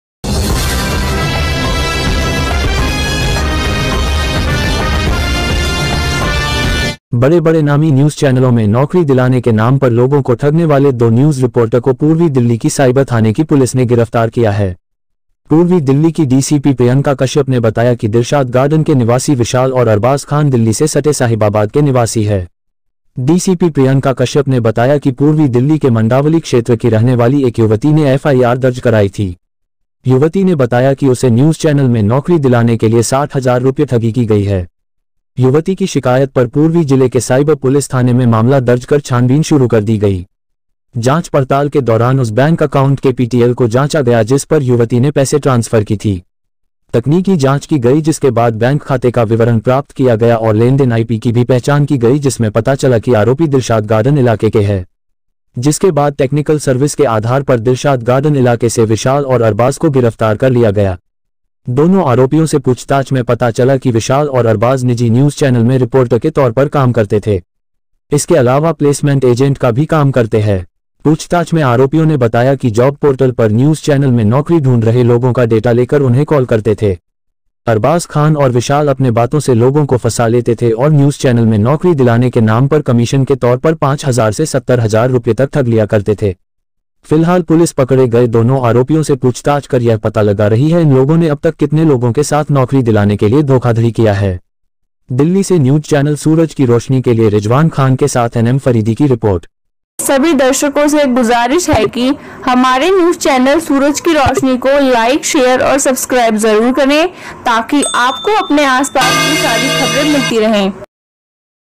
Media में नौकरी दिलाने के नाम से लोगों से ठगते थे लाखों रुपए East dehli पुलिस धरदबोचा
बड़े बड़े नामी न्यूज़ चैनलों में नौकरी दिलाने के नाम पर लोगों को ठगने वाले दो न्यूज़ रिपोर्टर को पूर्वी दिल्ली की साइबर थाने की पुलिस ने गिरफ्तार किया है।

डीसीपी प्रियंका कश्यप ने बताया कि दिलशाद गार्डन के निवासी विशाल और दिल्ली से सटे साहिबाबाद के निवासी अरबाज़ खान है।

डीसीपी ने बताया कि पूर्वी दिल्ली के मंडावली क्षेत्र की रहने वाली एक युवती ने एफ आई आर दर्ज कराई थी।

युवती ने बताया कि उसे न्यूज़ चैनल में नौकरी दिलाने के लिए 60 हजार रुपये ठगी की गई है।

युवती की शिकायत पर पूर्वी जिले के साइबर पुलिस स्टेशन में मामला दर्ज कर मामले की छानबीन शुरू की गई ।

जांच पड़ताल के दौरान उस बैंक अकाउंट केपीटीएल को जांचा गया जिस पर युवती ने पैसे ट्रांसफर किए था।

तकनीकी जांच की गई जिसके बाद बैंक खाते का विवरण प्राप्त किया गया और लेनदेन आईपी की भी पहचान की गई जिसमें पता चला कि आरोपी दिलशाद गार्डन इलाके के है।

जिसके बाद टेक्निकल सर्विस के आधार पर दिलशाद गार्डन इलाके से विशाल और अरबाज़ को गिरफ्तार कर लिया गया।

दोनो आरोपियों से पूछताछ में पता चला कि विशाल और अरबाज निजी न्यूज़ चैनल में रिपोर्टर के तौर पर काम कर रहा थे साथ ही प्लेसमेंट एजेंट का भी काम करता था ।

पूछताछ में आरोपी ने बताया कि जॉब पोर्टल पर न्यूज़ चैनल में नौकरी ढूंढ रहे लोगों का डेटा लेकर उनहे कॉल कर थे।

अरबाज़ खान और विशाल अपने बातों से लोगों को फसा लेते थे और न्यूज़ चैनल में नौकरी दिलाने के नाम पर कमीशन के नाम पर 5000 से 70 हजार रुपये तक ठग लिया कर थे।

फिलहाल पुलिस पकड़े गए दोनो आरोपियों से पूछताछ कर यह पता लगा रही है इन लोगों ने अब तक कितने लोगों के साथ नौकरी के नाम से धोखाधड़ी किया है

दिल्ली से न्यूज चैनल सूरज की रोशनी के रिजवान खान के साथ एनएम फरीदी की रिपोर्ट